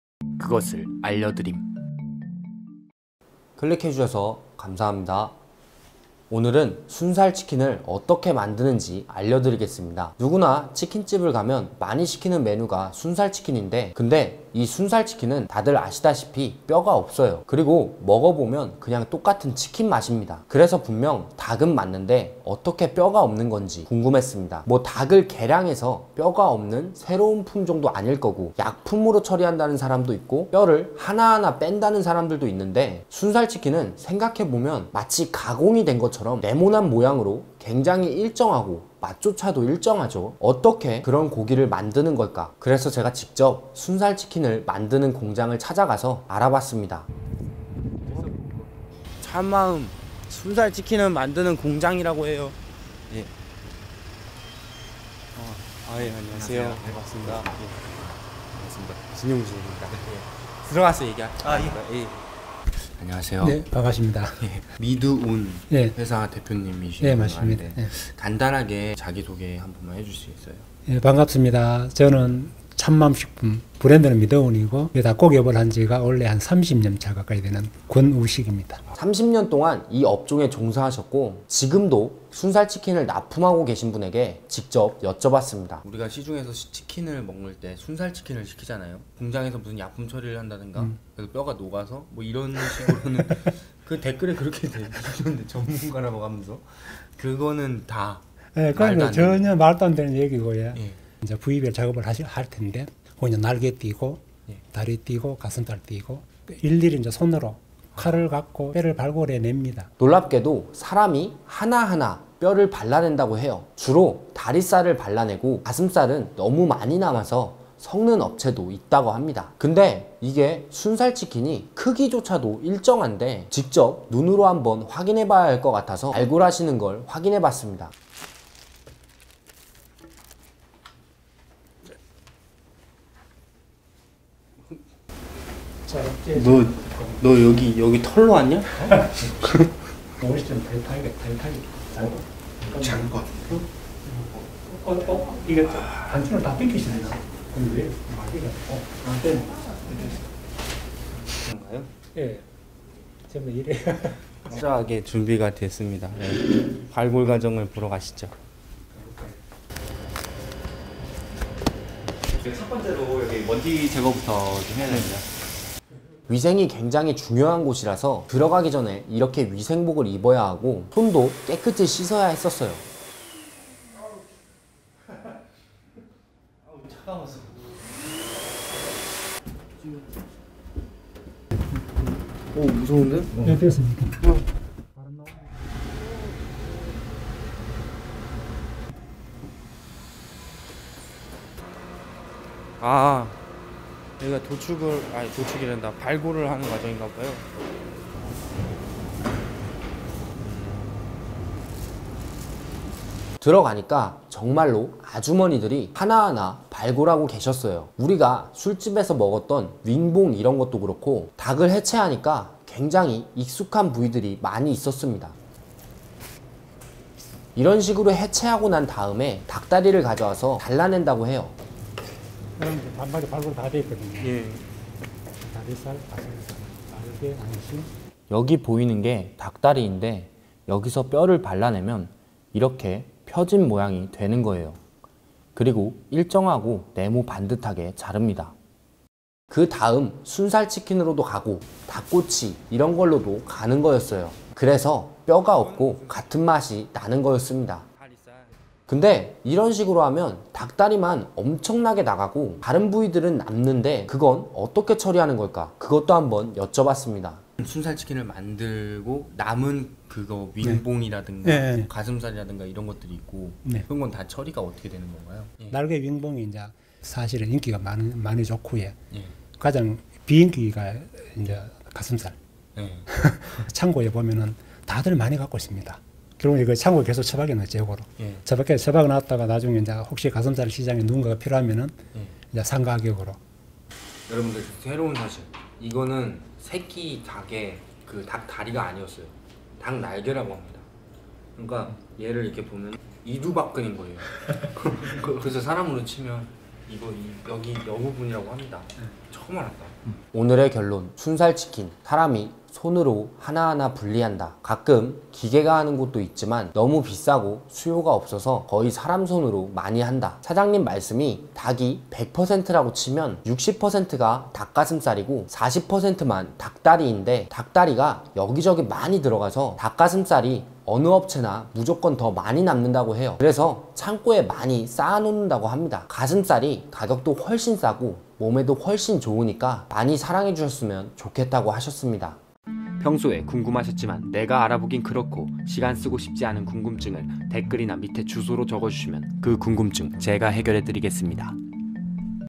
순살치킨은 어떻게 만드는 걸까?
치킨은 분명히 뼈가 있는데
제가 직접 공장에가서 알아봤습니다.

협조 : 미드운 참마음식품 권우식 대표님, 권대욱님
촬영 : 신연지, 최정진
편집 : 이희태
기획 : 진용진, 신연지